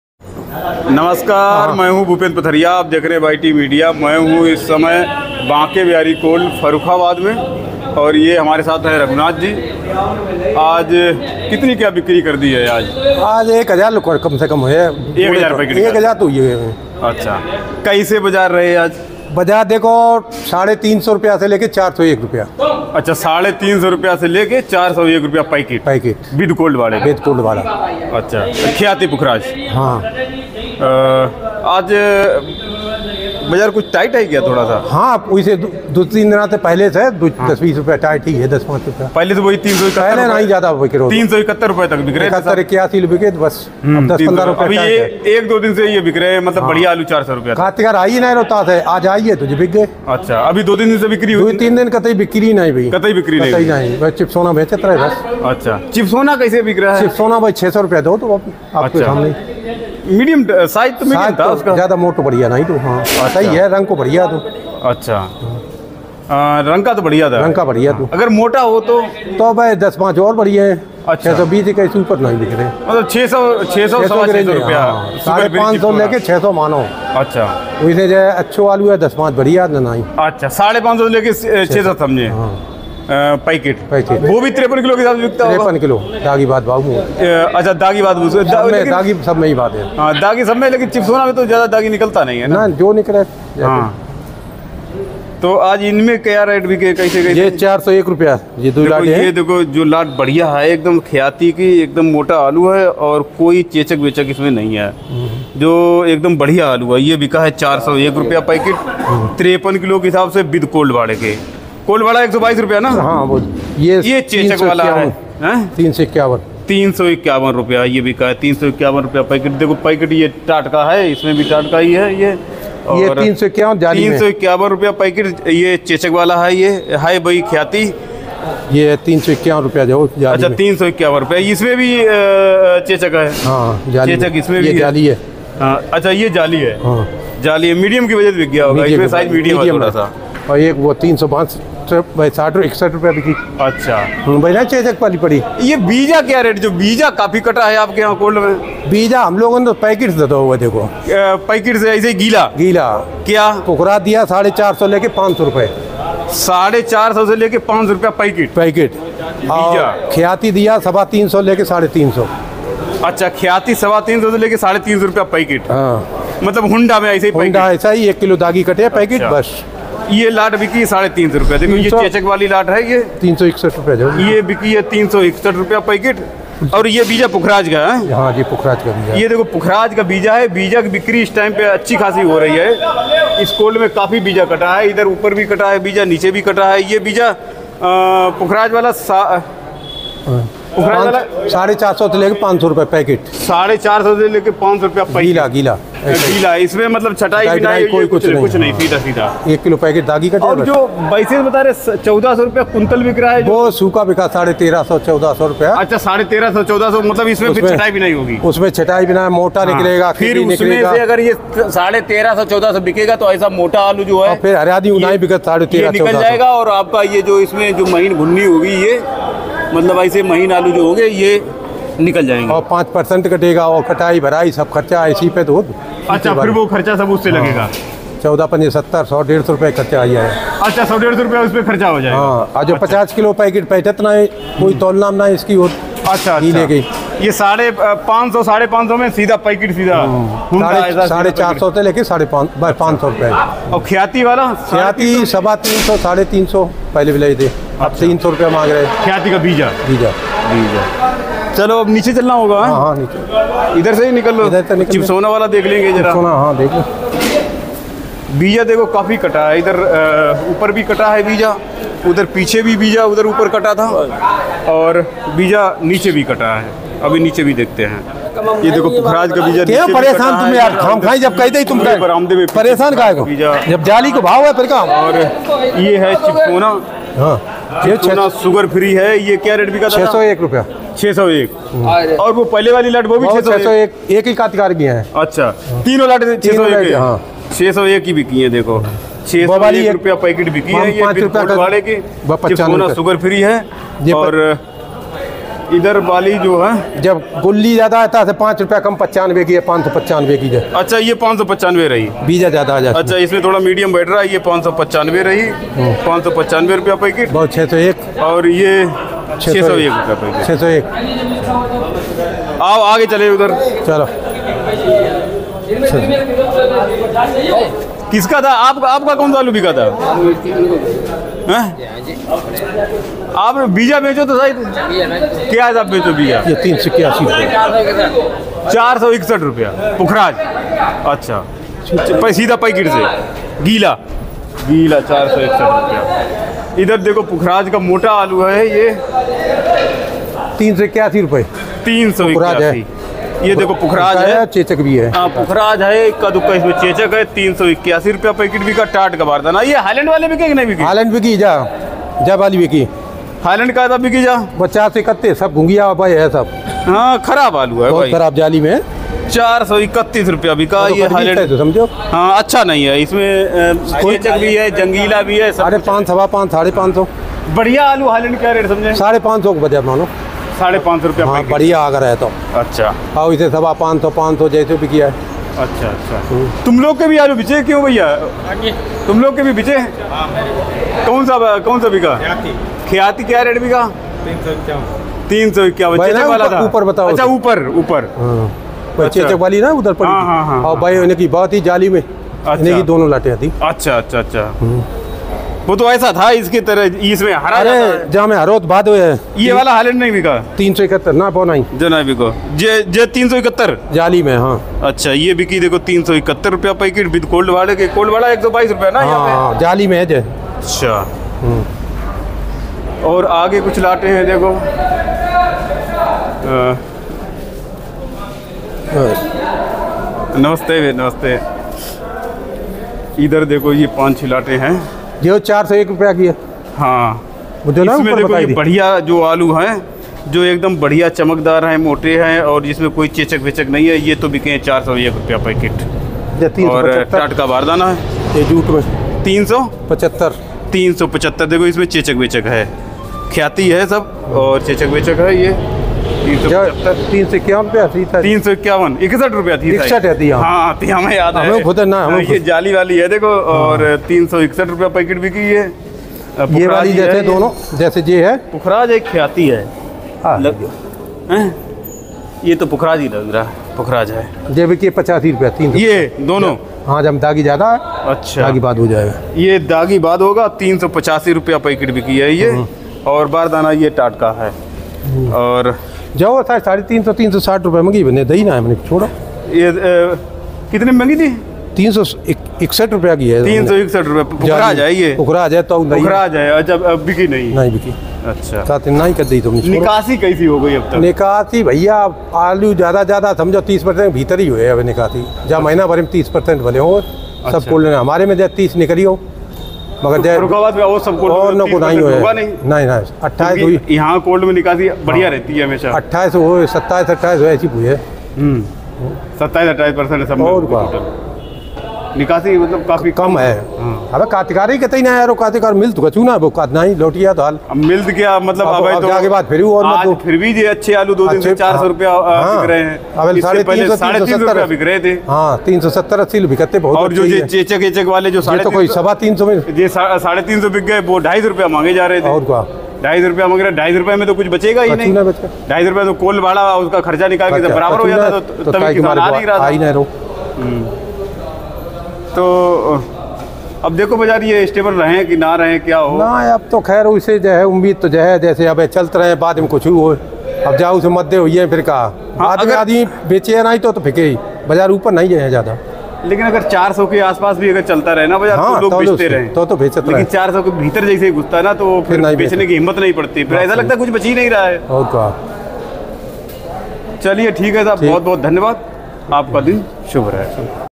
25 अगस्त 2023/ आलू के भाव में दिखा सुधार देखें क्या रेट बिके आज आलू
Aalu ka aaj ka bhav Banke Bihari cold Farrukhabad